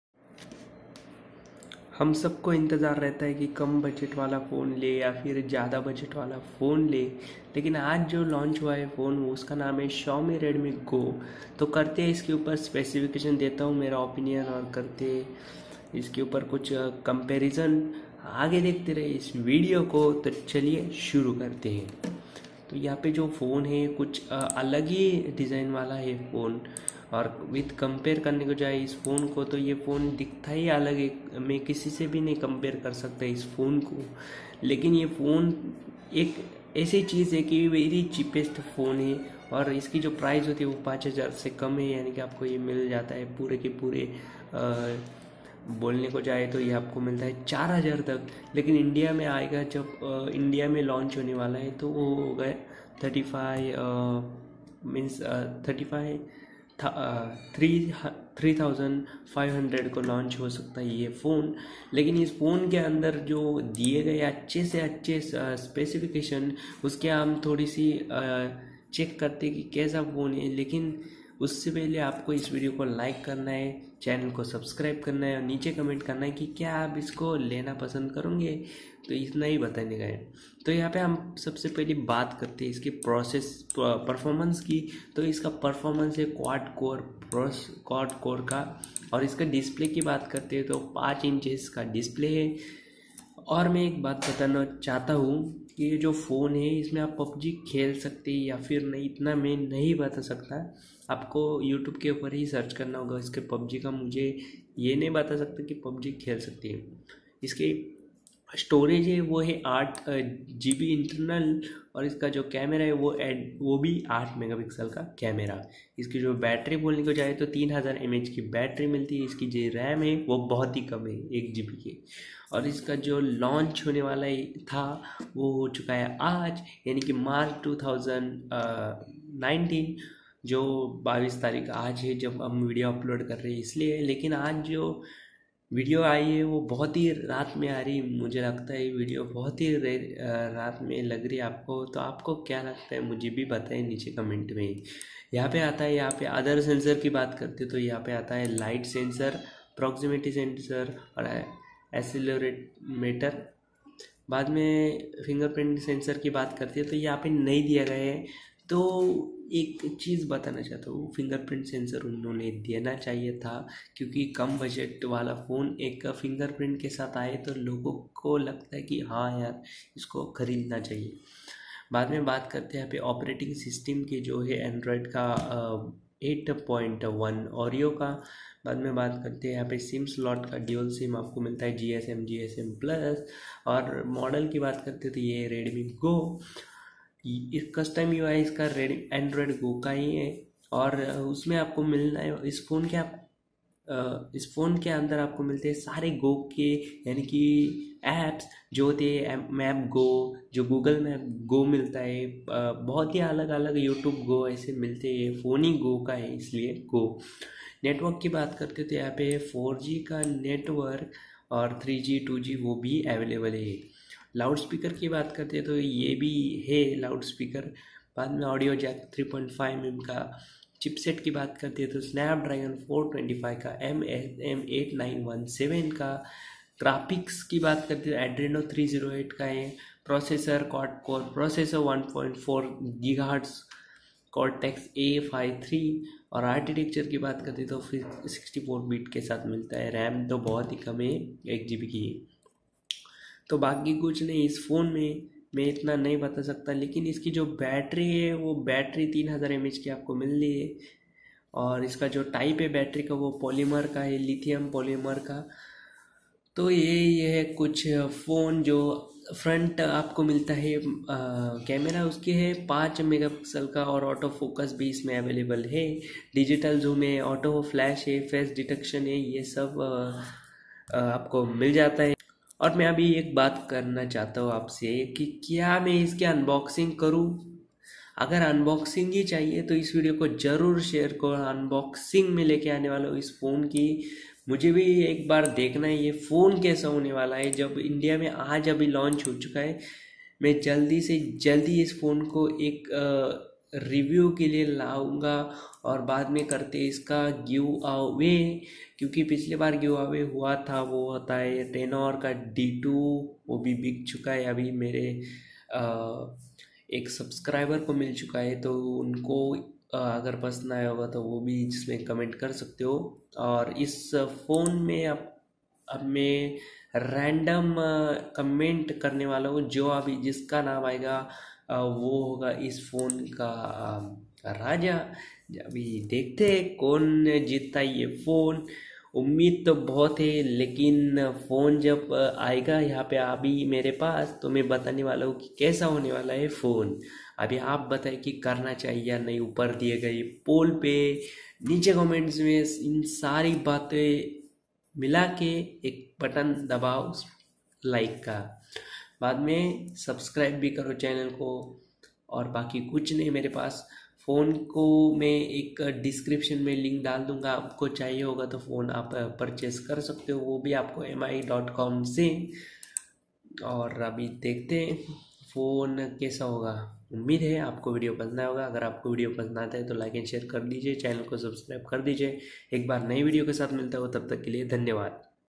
Xiaomi Redmi Go launched in India:specs, top features, India price, and everything you need to know
Xiaomi Redmi Go mobile features a 5.0" (12.7 cm) display with a screen resolution of HD (720 x 1280 pixels) and runs on Android v8.1 (Oreo) operating system. The device is powered by Quad core, 1.4 GHz, Cortex A53 processor paired with 1 GB of RAM. As far as the battery is concerned it has 3000 mAh. Over that, as far as the rear camera is concerned this mobile has a 8 MP camera . Other sensors include Light sensor, Proximity sensor, Accelerometer. So, does it have a fingerprint sensor? No, it doesn't. For graphical performance that can make games run smoothly, this phone has got a Adreno 308 GPU. On board storage is at 8 GB with the option to expand the memory by Yes Up to 128 GB. Design is one of the most important factors when it comes to mobiles. This phone is 8.3 mm slim and weighs 137 grams.